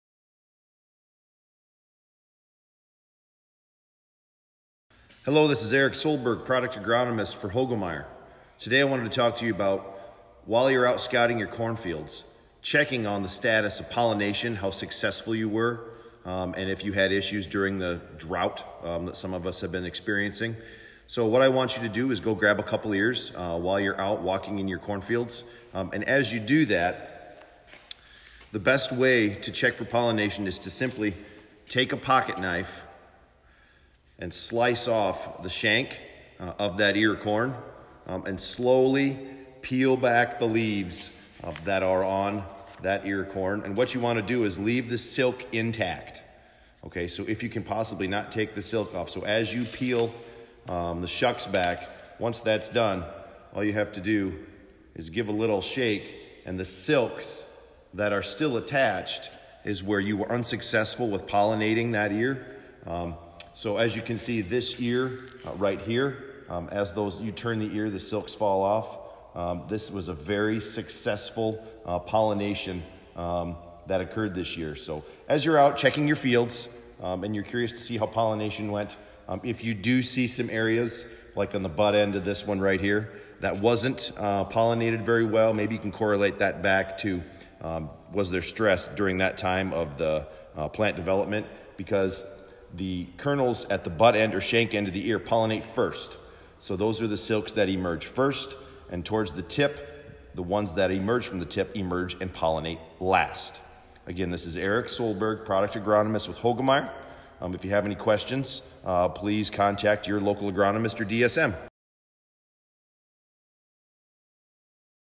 Corn Pollination and Grain Fill
Corn pollination has been successfully completed in many areas throughout the Western Corn Belt. Learn from Hoegemeyer product agronomist, Eric Solberg, on how to identify successful corn pollination.